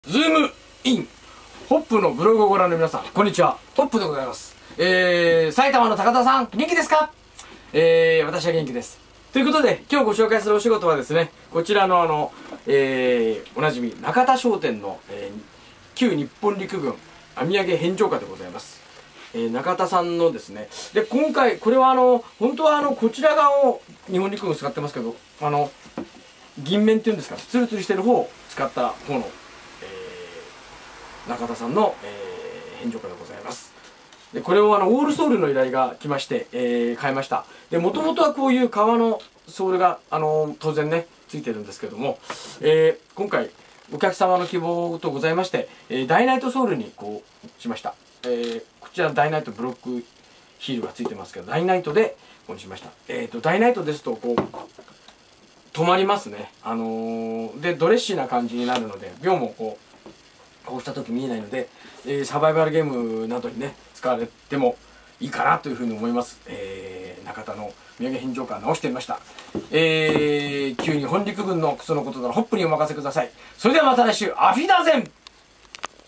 旧日本陸軍編上げ靴修理
中田商店製の旧日本陸軍編上げ靴のソールを交換しました。